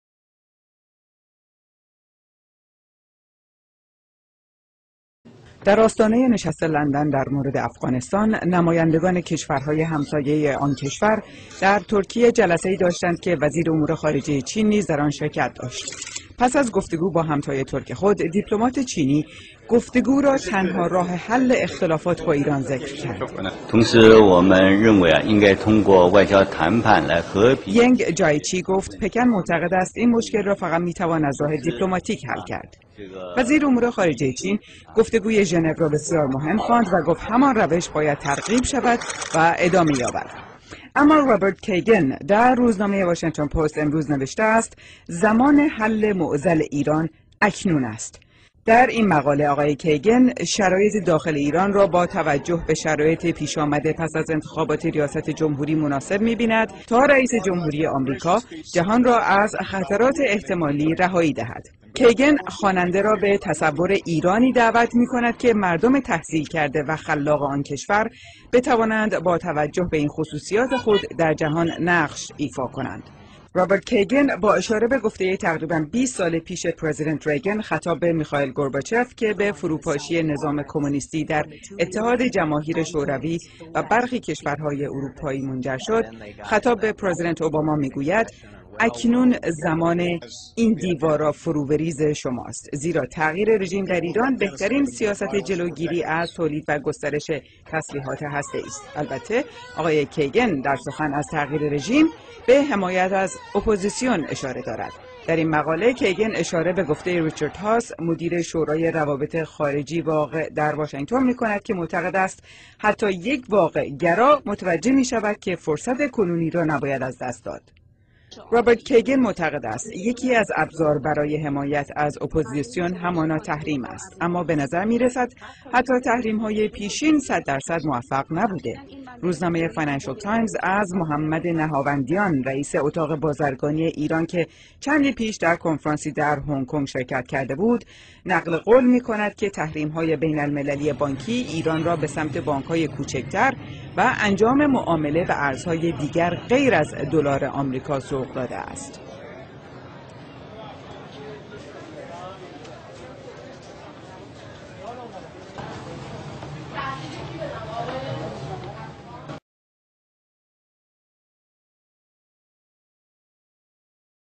گزارشی از نشست ترکیه و مسایل ایران
گزارش جدیدی از سازمان جهانی گزارشگران بدون مرز درباره نشست ترکیه و حمایت دوباره نماینده "چین" از رژیم ملایان.